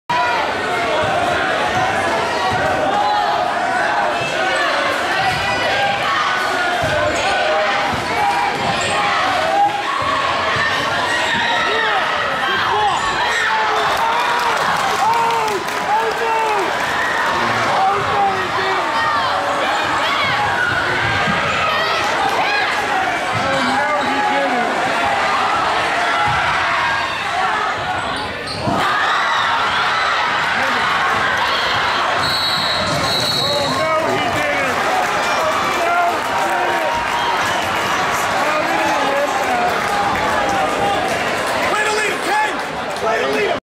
Caden Gigstad vs Chatfield OT to tie the game
Caden Gigstad #2 hits 3-pointer in OT vs Chatfield (1/24/2020) to tie the game.